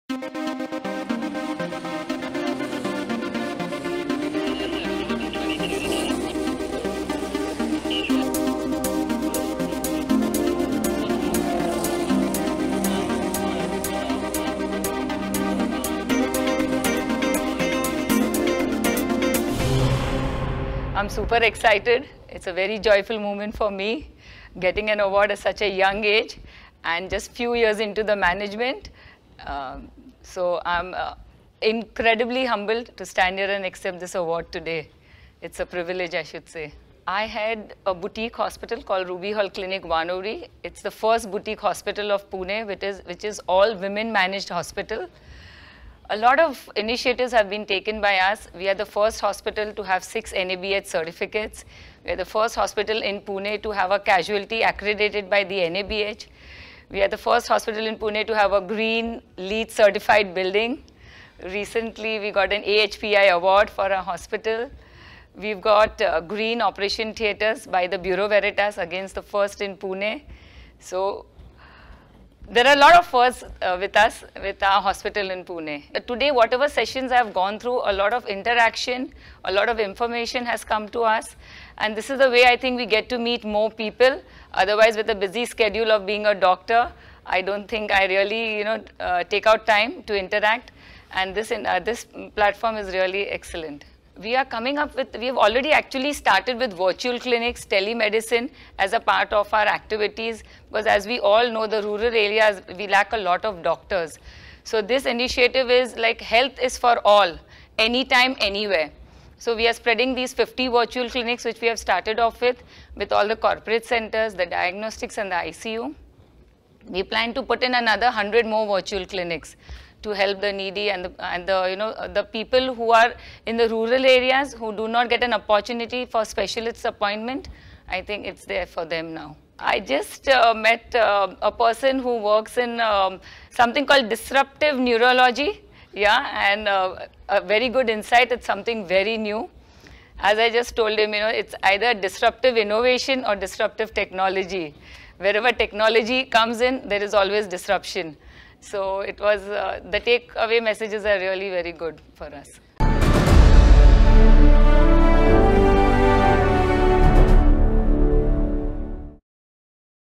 Dr. Manisha Karmarkar, COO, Ruby Hall at the 3rd Icons of Healthcare Summit, Singapore 2018
Dr. Manisha Karmarkar, COO Ruby Hall Clinic Wanowrie, Pune was presented the "Healthcare Women Icon" Award 2018 by BERG Singapore at the 3rd Icons of Healthcare Summit & Awards, Singapore.
With the economics of healthcare shifting from growth-mode to achievement of efficiencies and value creation, Dr.Manisha leads Ruby Hall Clinic Pune’s commitment to patient-centric, holistic and compassionate healthcare.  The clinic is also the only all-women managed multi-speciality tertiary care medical centre in India.